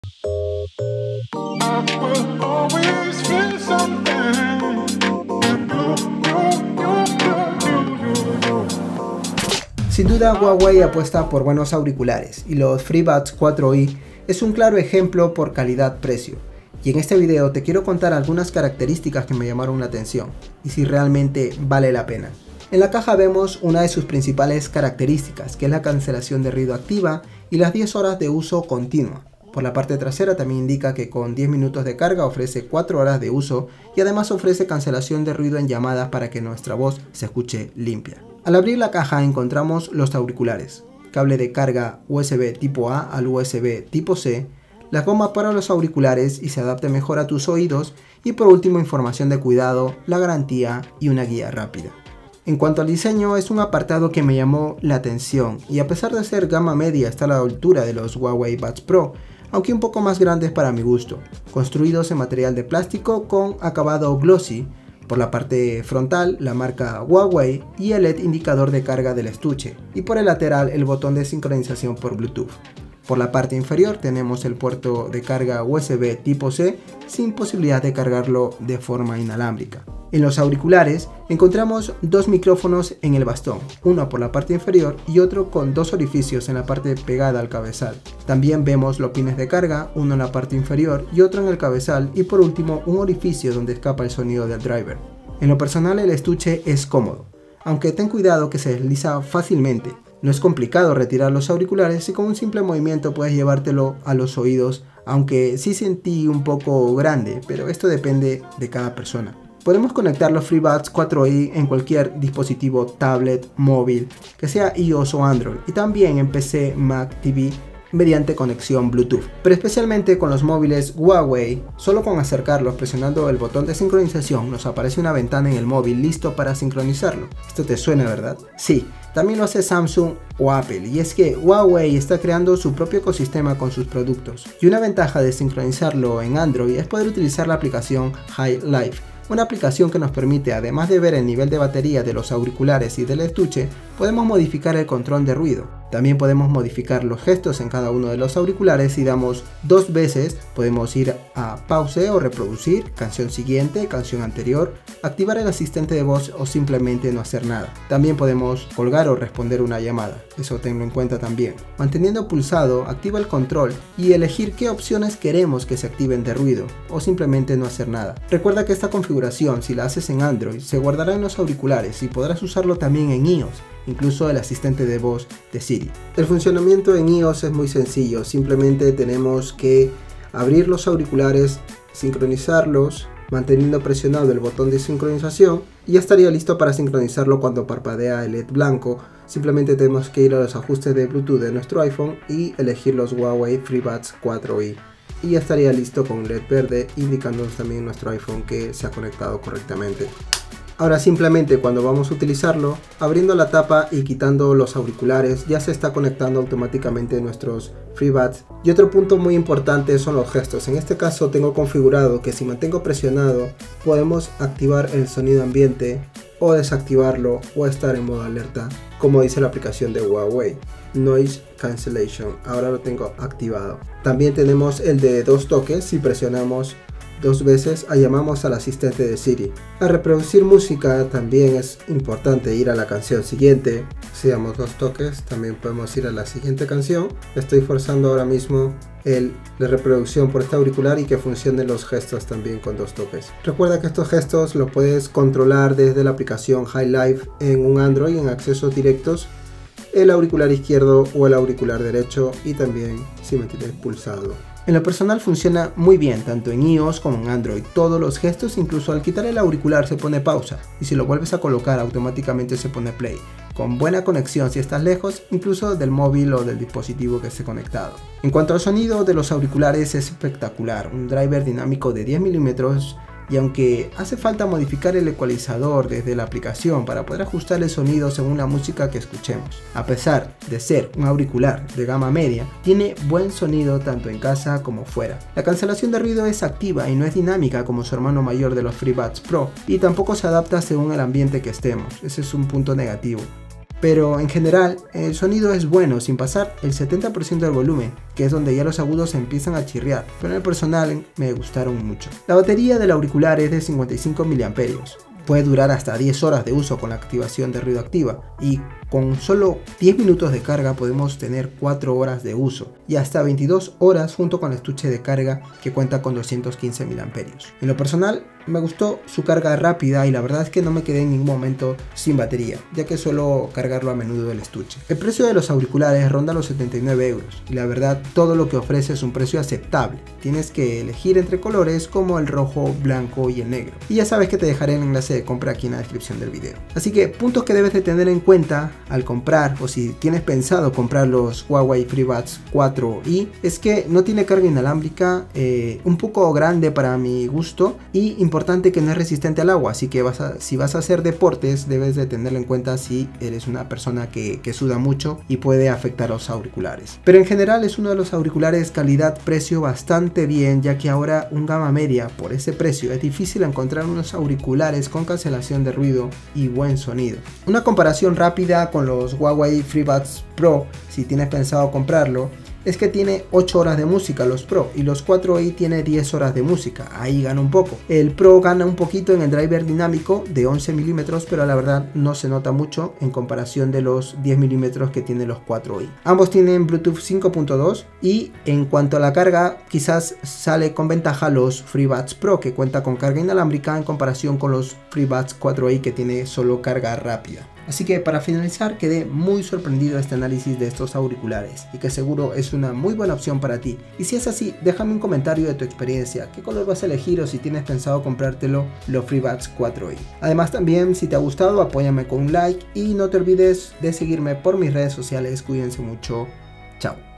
FreeBuds 4i: Análisis y experiencia + Configuración + Instalación [Android - IOS]
Comprar en amazon

00:00 Comienzo
00:39 Unboxing
00:53 Diseño estuche
01:27 Diseño auriculares
01:34 Experiencia diseño
01:49 Compatible con android - iOS ?
01:59 Configurar en Huawei
02:17 Configurar Freebuds 4i en AI Life en (android menos iOS)
02:55 Configurar en IOS
03:32 Gestos de FreeBuds 4i en iOS y Android
03:48 Llamar a SIRI en FreeBuds 4i
04:19 Configurar Play/Pause en iOS - Android
04:27 Sonido
05:02 Bateria
05:25 Precio
05:37 Puntos importante (Resistente al agua ?)
06:11 Comparación Freebuds 4i vs Huawei FreeBuds Pro
06:39 Final